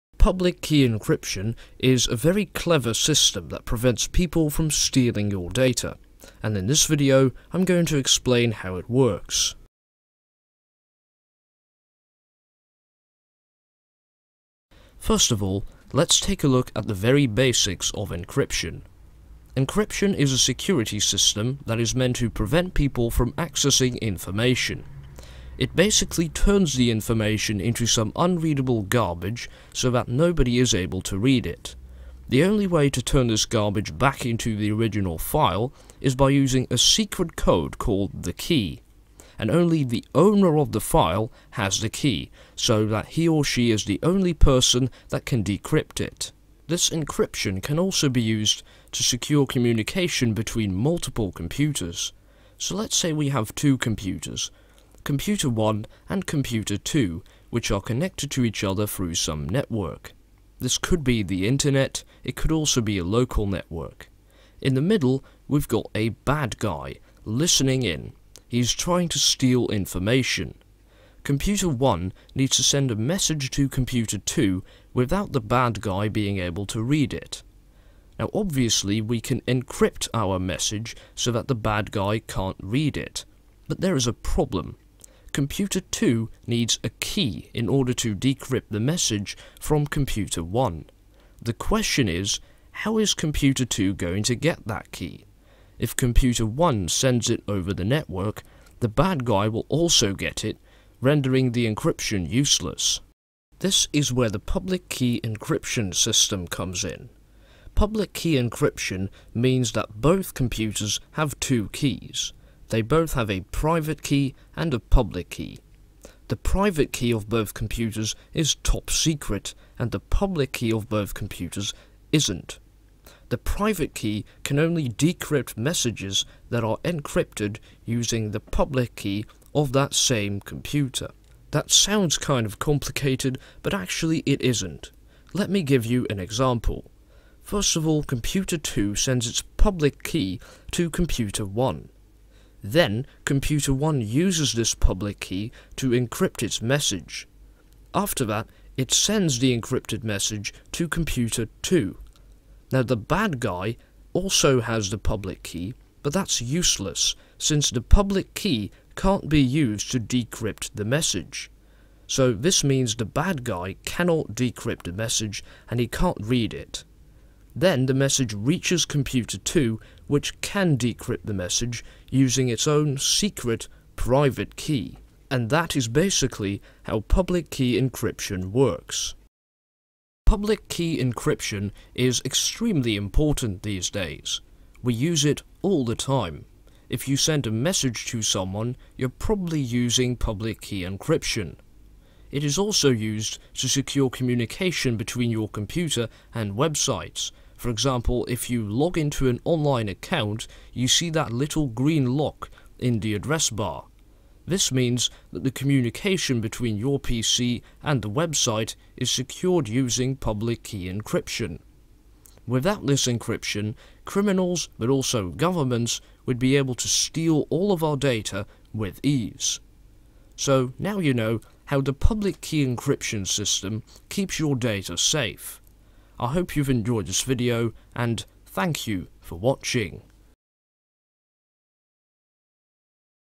How does public key encryption work - simplified (AKIO TV)
This is a basic explanation of public key encryption. Public key encryption allows for secure communication over the internet, which is why it's so extremely important these days. I hope you enjoy watching this video!!

(AKIO TV) MMXVII